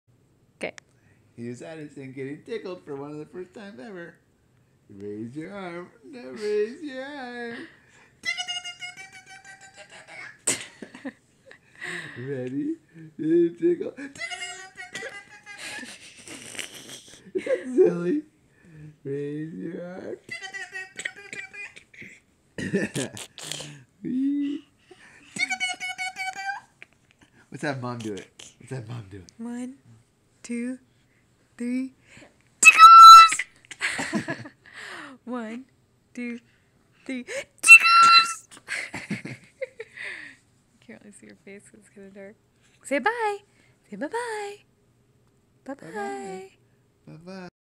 Mar 21, 2010 8:28 PM
Addison getting tickled